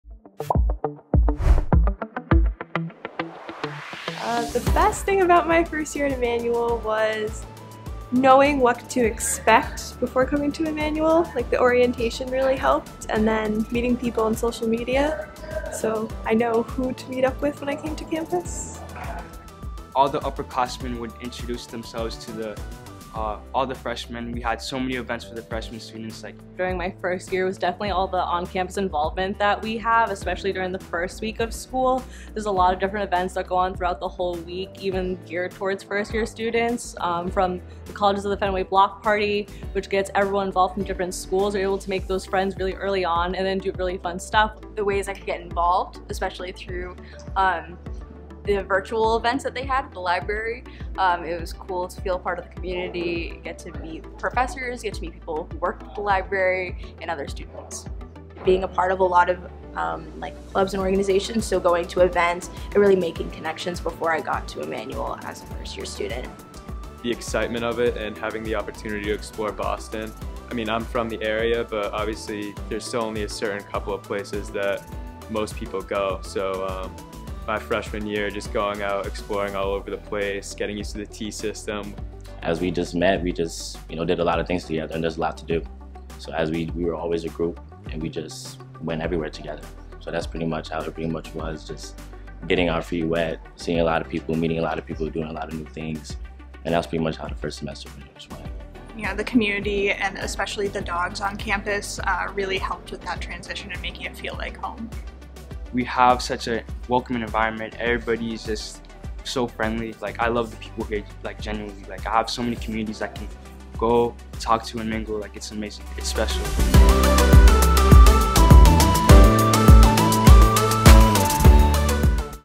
What to Expect from Your First Year at Emmanuel
It's a common question from any first-year student: What can I expect from my first year at college? For Emmanuel students, the first few days on campus are filled with excitement and countless opportunities to get involved. You'll be met with equal enthusiasm from our community, who are just as excited to welcome you to the Saints family.